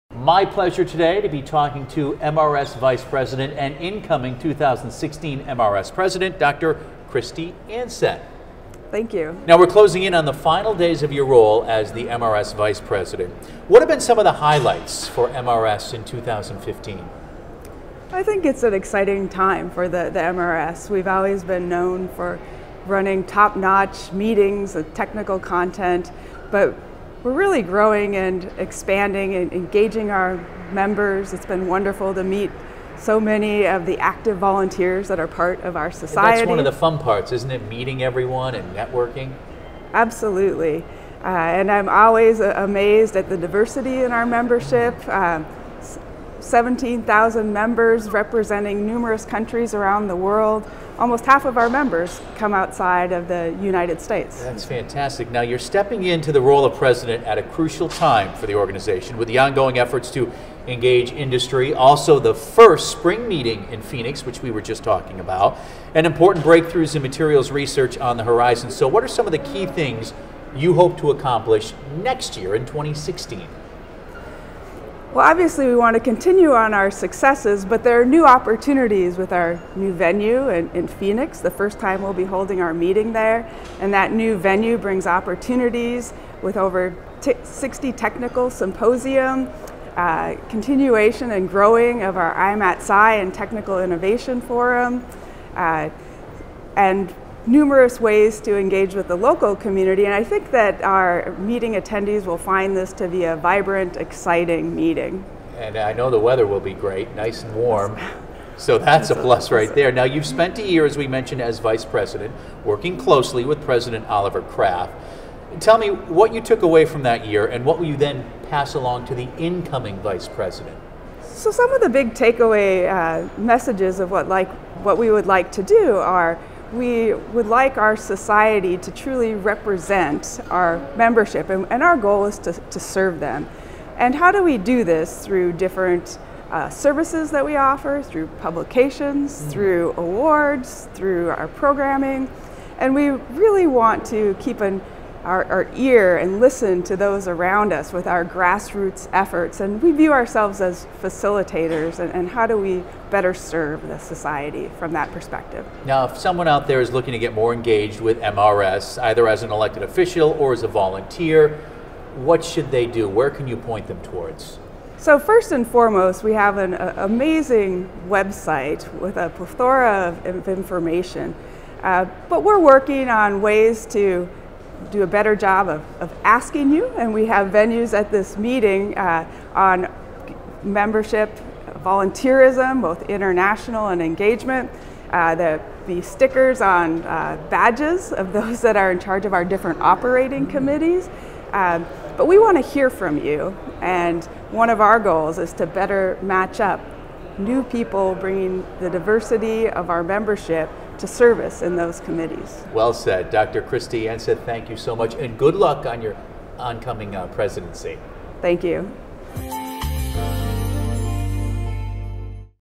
MRS Vice President and 2016 MRS President Kristi Anseth
MRS Vice President Kristi Anseth discusses her plans as 2016 MRS President with MRS TV.